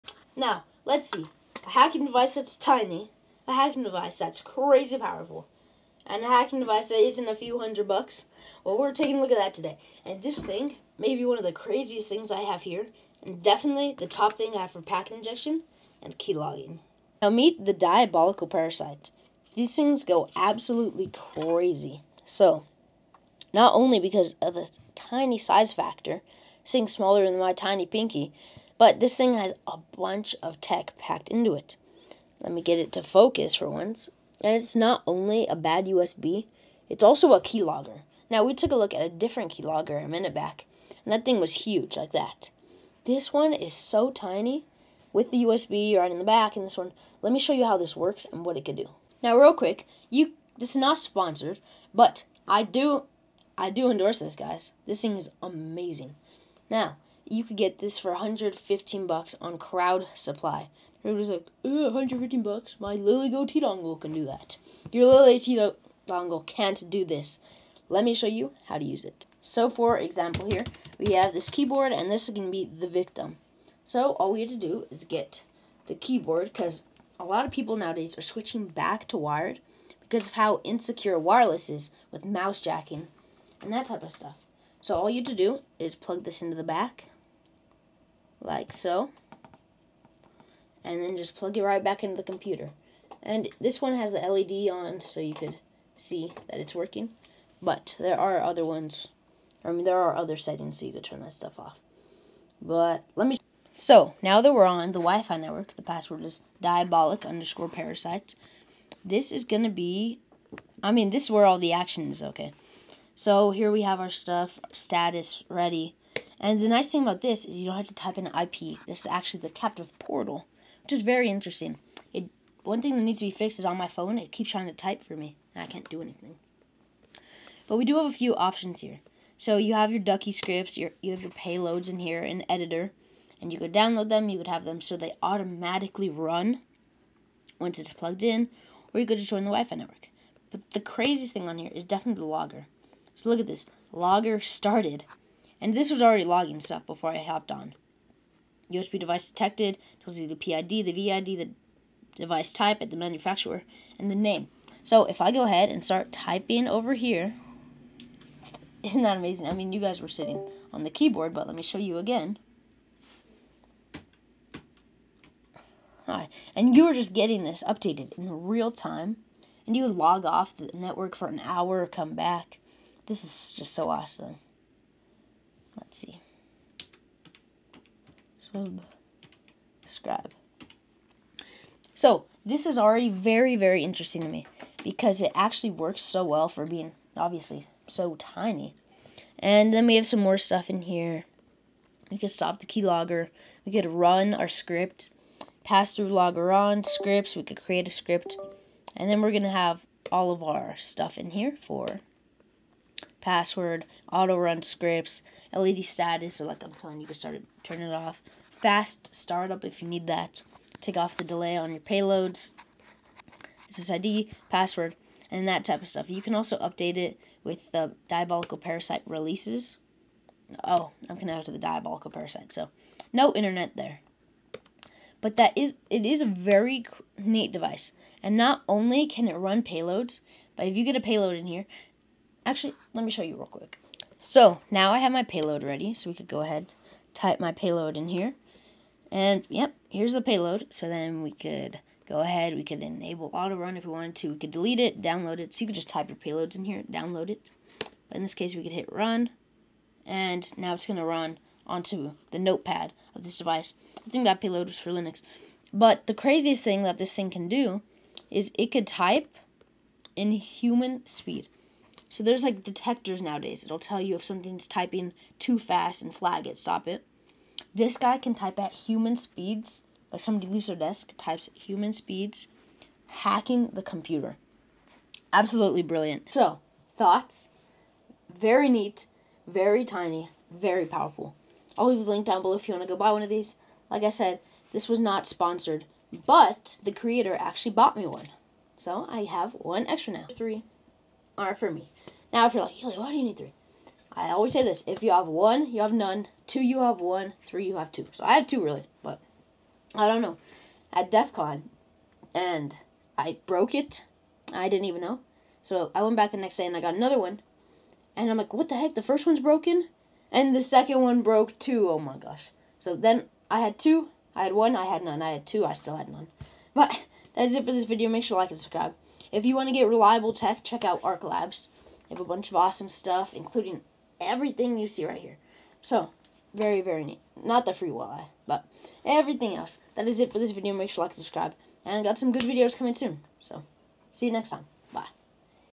The Diabolic Parasite – Injection and Keylogging in One Device!
The Diabolic Parasite is one of the most insane payload-injection and keylogging tools I’ve ever tested. This tiny device can run stealthy attacks, automate payloads, and capture keystrokes like nothing else out there. It’s built for precision, power, and total control — all in a form factor that looks deceptively simple.

In this video, I break down how it works, what makes it different, and why it’s one of the coolest tools I’ve added to my hacking setup yet.